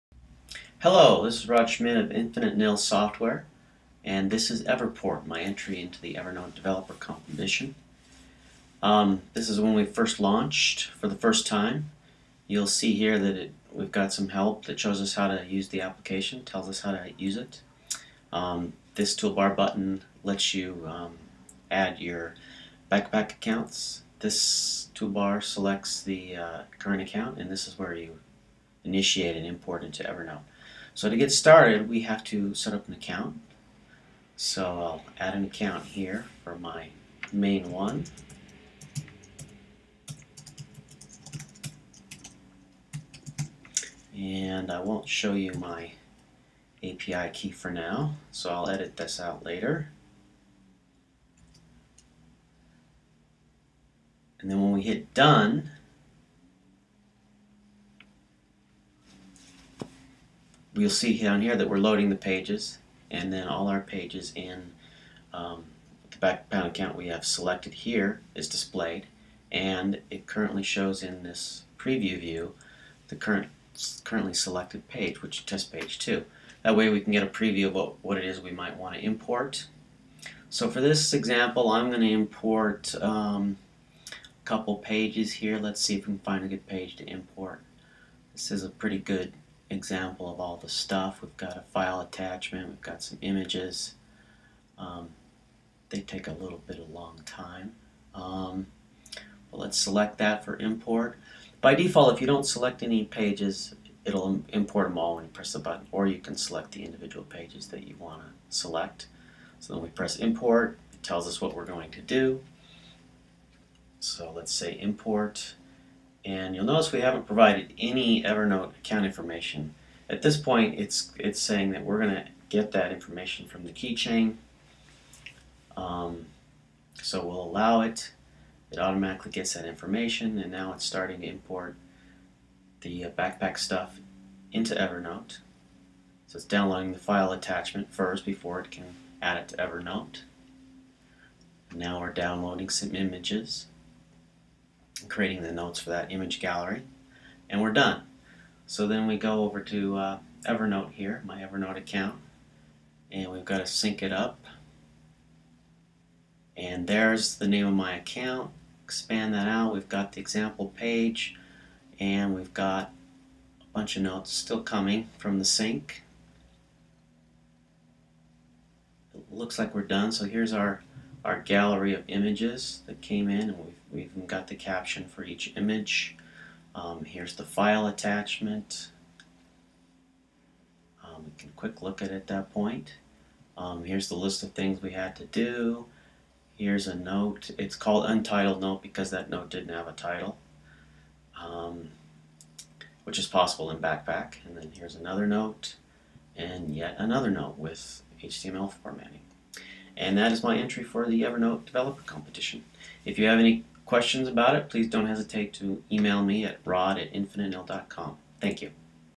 Everport
Everport is my entry into the Evernote Developer's Competition and a new product of infiniteNIL. It imports data from Backpack to Evernote.